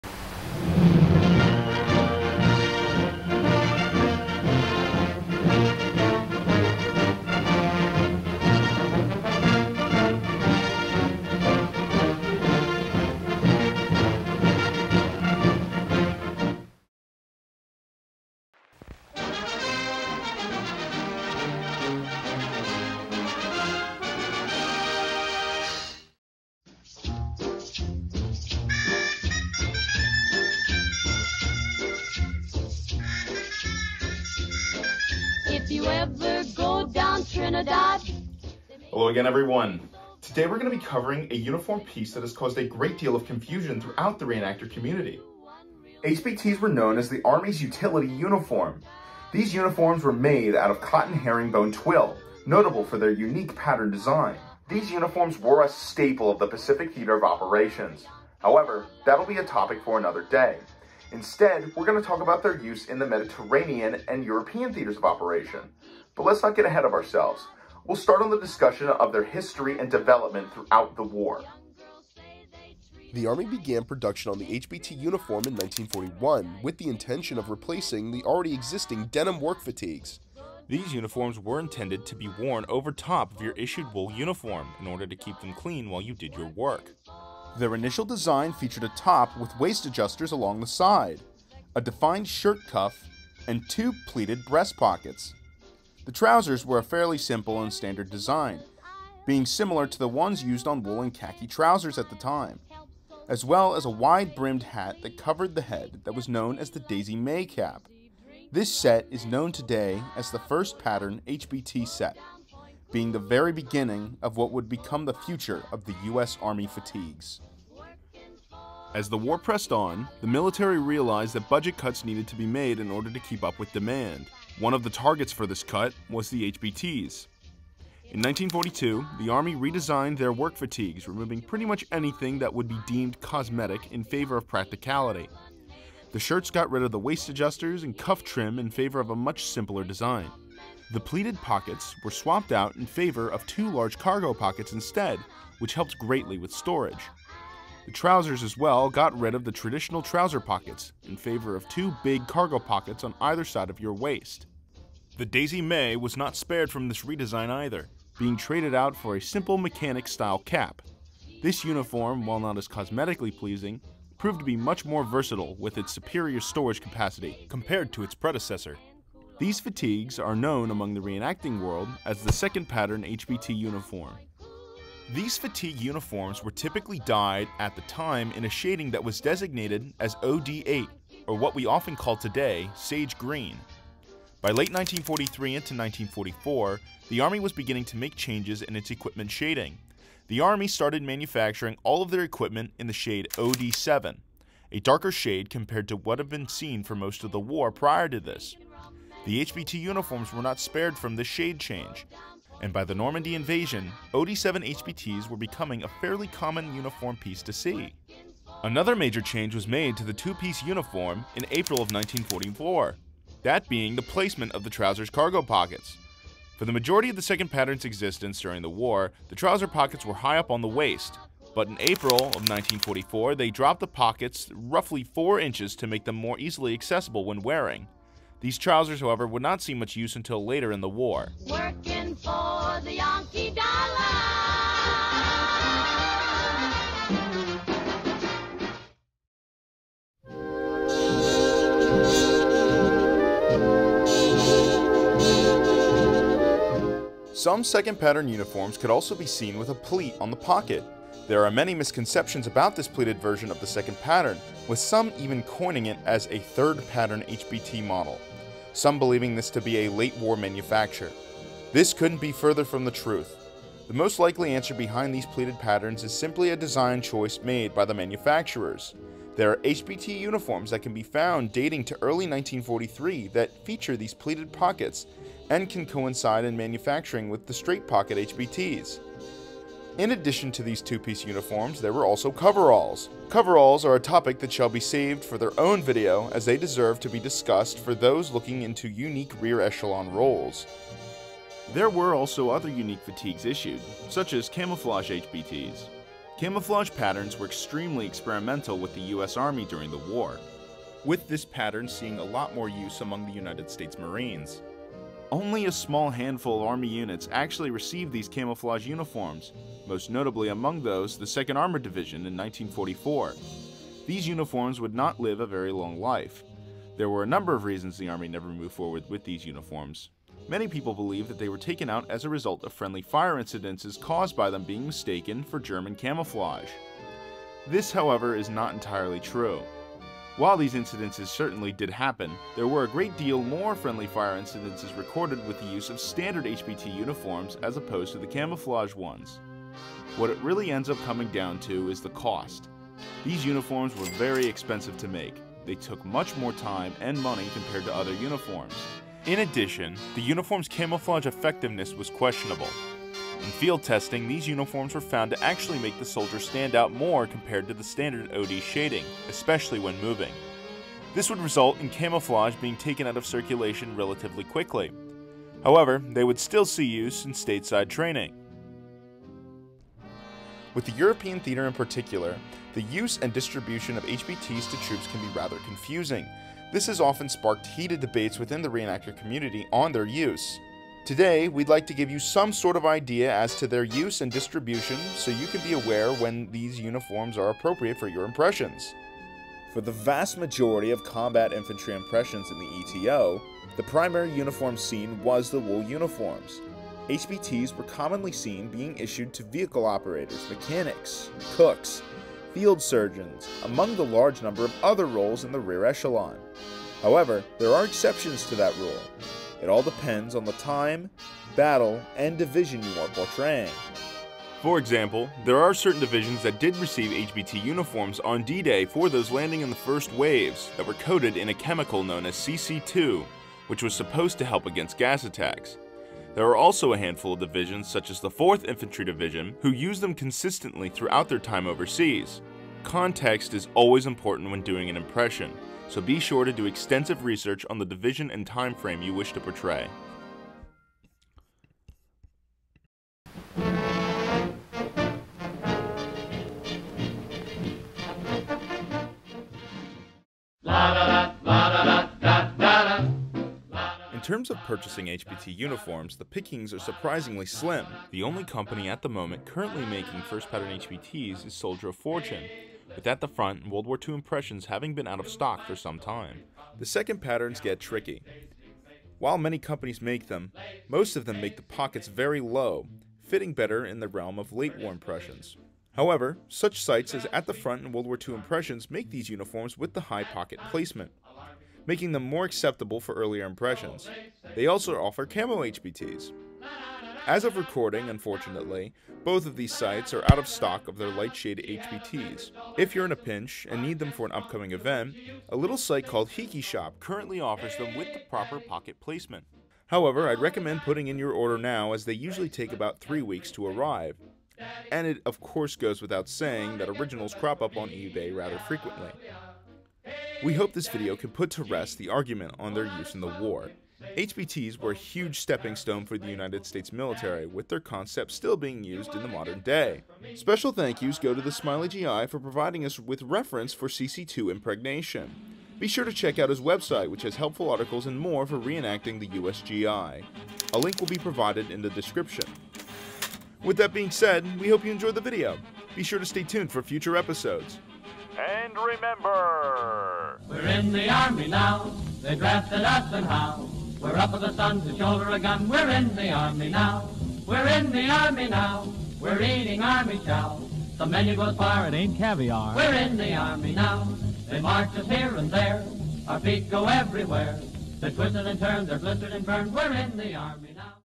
Lessons in Reenacting | Ep. 14 | HBT Uniforms
Were they used by the front lines? Rear Echelon? Maybe Both? HBTs can be a big anomaly for reenactors. Today's episode will try to answer that for you! Watch as we delve deep into their creation and development throughout World War II!

Special Thanks to the Smiley GI for providing us with CC2 examples as well as At the Front for their informative article on HBTs that helped make this video!